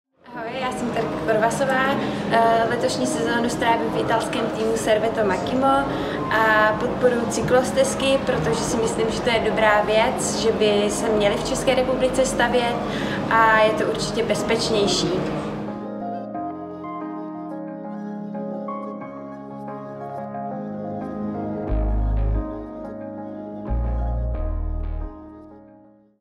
Cyklovize 2030 - Tereza Korvasová
🚴‍♂️ Dalším ambasadorem Cyklovize 2030 není nikdo jiný než dvojnásobná mistryně republiky v časovce jednotlivců - Terka Korvasová.

🇨🇿🇮🇹 Členka reprezentačního týmu ČR a Italského UCI týmu Servetto Makhymo Beltrami TSA by si v Česku přála více bezpečných cyklostezek.

👋 Jak můžete Terce s tímto nelehkým úkolem pomoci? Obraťte se na svoje město/obec s malým dotazem a jednou prosbou:
„Kolik kilometrů cyklostezek chcete zrealizovat do konce roku 2023 a kolik přibližně budou stát“?